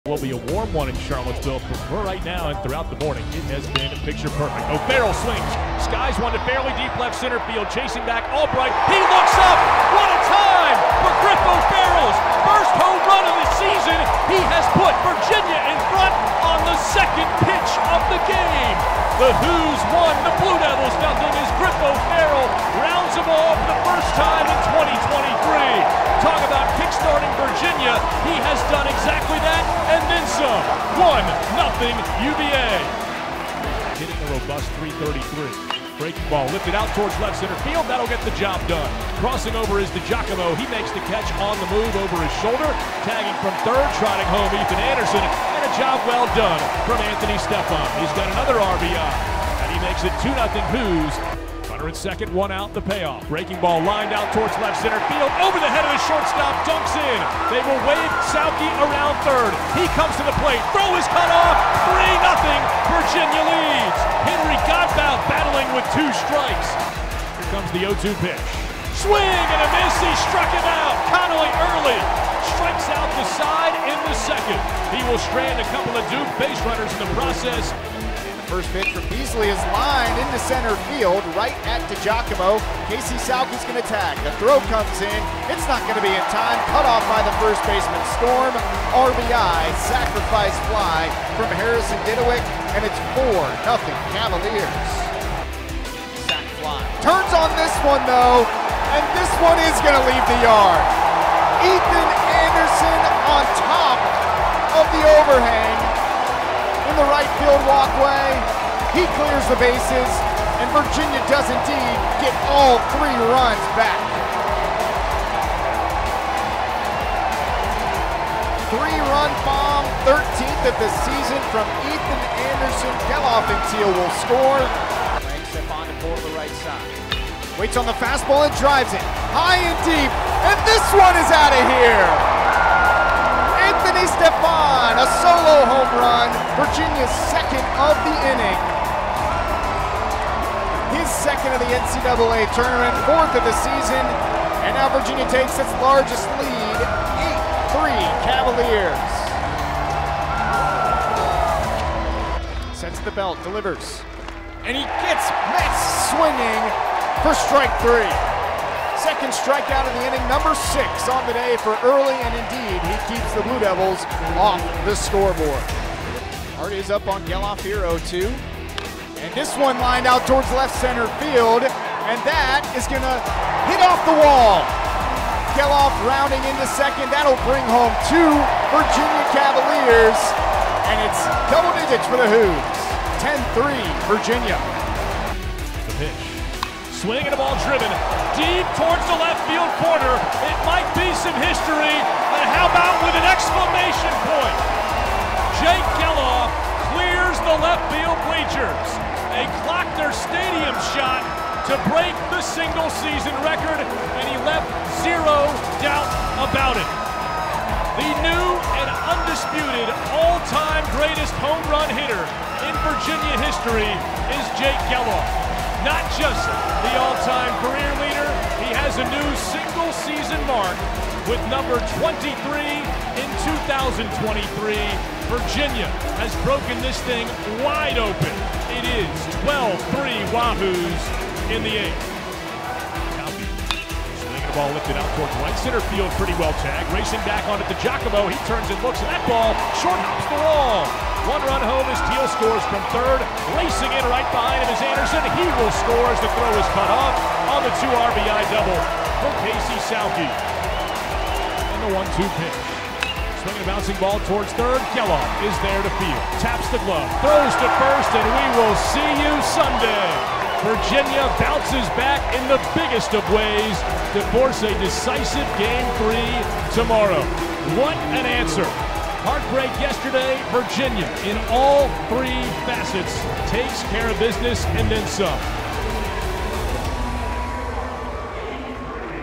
BASEBALL: Duke vs. Virginia (Super Regional Game 2) Highlights
The Wahoos answer back and dominate game two against Duke to force game three.